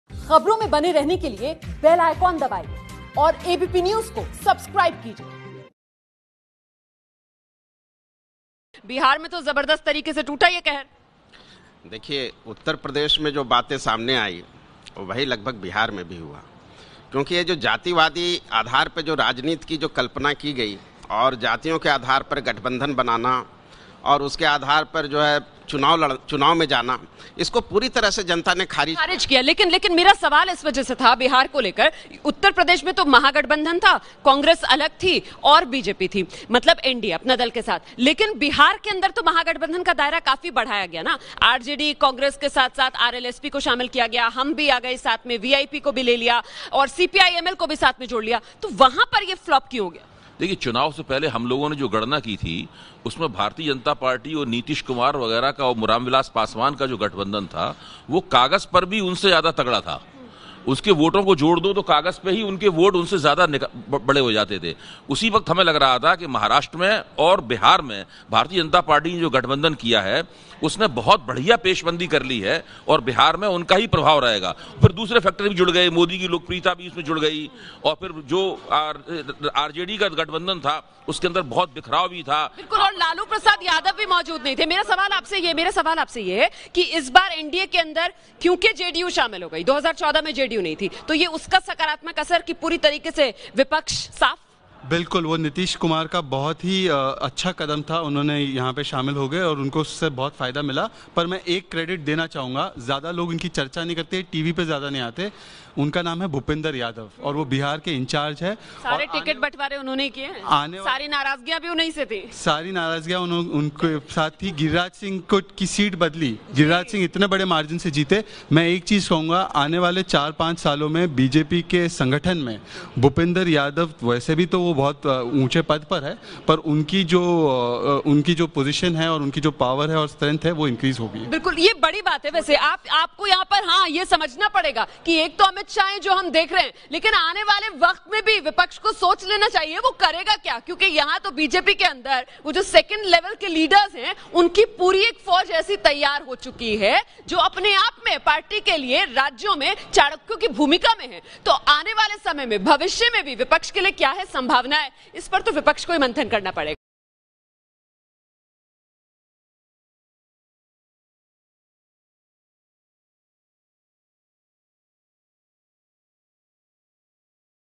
बिहार भी हुआ मोदीमय, नीतीश कुमार के साथ होना का कितना हुआ असर ? जानिए एक्सपर्ट्स से
एक और जहां विरोधियों के बीच एक एक सीट को लेकर घमासान मचा हुआ था वहीं बीजेपी ने बिहार में अपने सहयोगियों का पूरा ख्याल रखा और पिछली बार 2 सीट पर सिमटने वाली जेडीयू को अपने बराबर 17 सीट दी और एलजेपी को 6 सीट दी, जबकि पिछली बार खुद बीजेपी 22 सीट पर जीती थी लेकिन इस बार उसने सहयोगियों के लिए अपनी जीती 5 सीट भी छोड़ दी. बीजेपी को इस त्याग का फल नतीजों में भी मिला और बिहार में एनडीए का गठबंधन पिछली बार से ज्यादा सीट जीतने में कामयाब रहा, वहीं विपक्षी गठबंधन चुनाव एलान के ठीक पहले तक सीट बंटवारे में ही उलझा रहा.